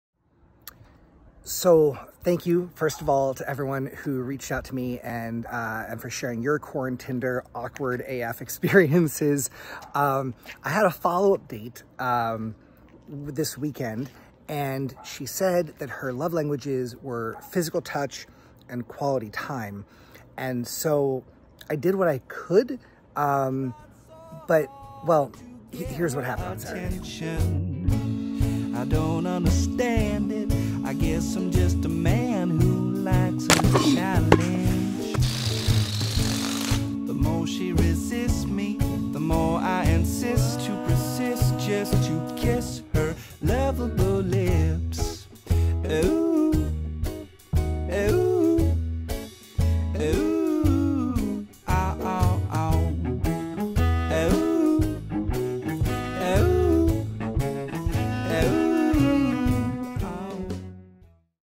Lovable Lips - Teaser #2
For those of you following my pandemic dating experience ... here's the follow-up with what happened this past weekend. She told me her love languages were physical touch and quality time ... so i did what I could .